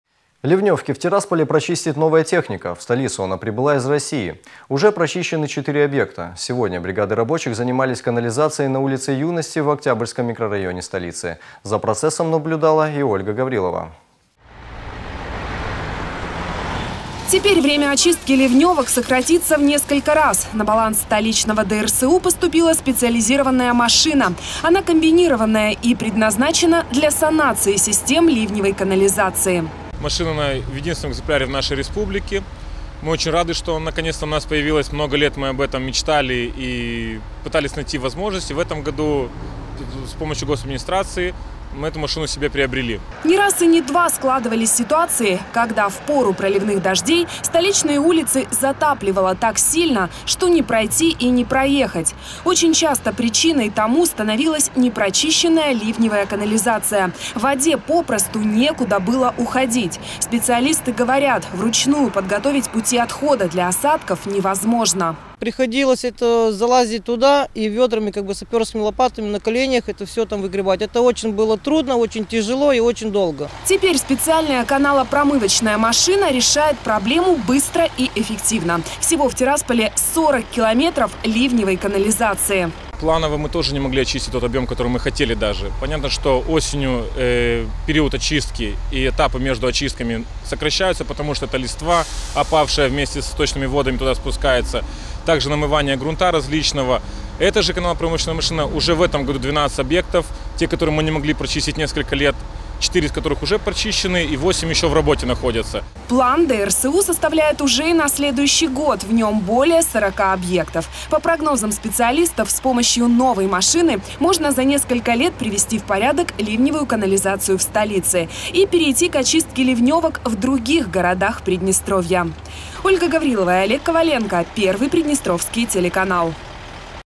В Тирасполе появилась машина для чистки ливневых канализаций
Ливневки в Тирасполе прочистит новая техника. В столицу она прибыла из России. Уже прочищены 4 объекта. Сегодня бригады рабочих занимались канализацией на улице Юности, в Октябрьском микрорайоне столицы.